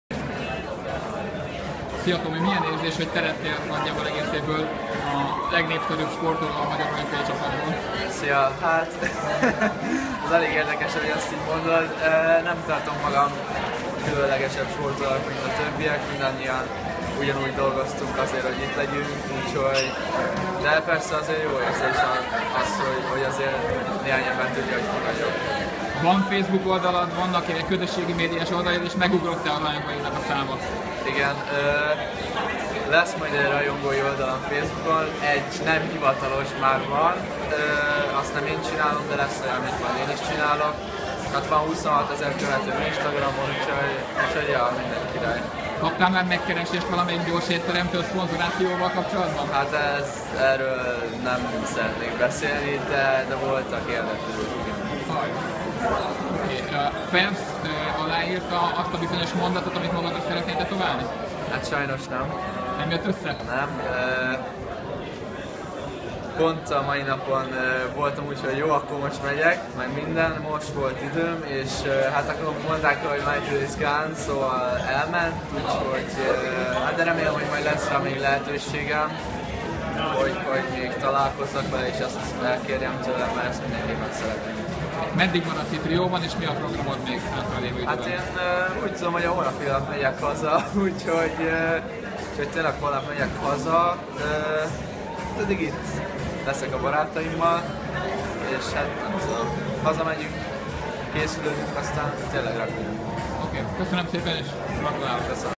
Kenderesi Tamás, az új magyar hős!
Kenderesi Tamással beszélgettünk Rióban!